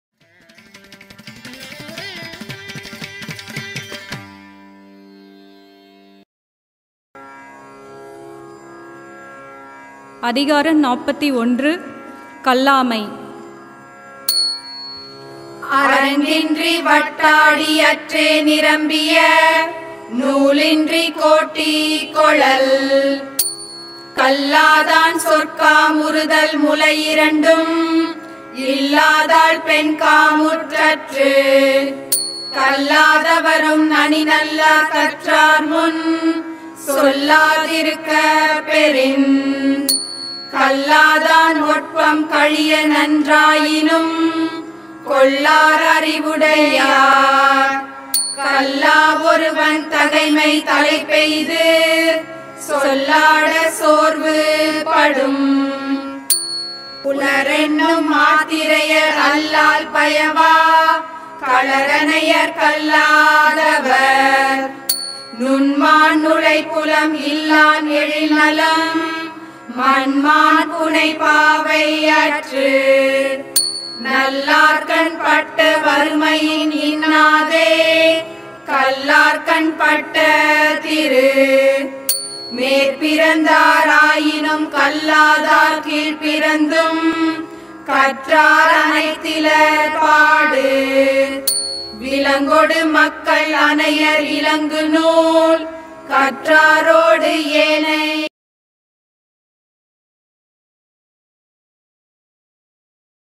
கல்லாமை || திருக்குறள் - அதிகாரம் 41 || Ignorance in Thirukkural || திருக்குறள் மறையோதல்
திருக்குறள் மறையோதல்:
அதிகாரம் 41: கல்லாமை   (Kallaamai )
Ignorance in Thirukkural

நன்றி:
முனைவர். சுந்தரவடிவேல் பாலசுப்ரமணியன்,
பனைநிலம் தமிழ்ச்சங்கம்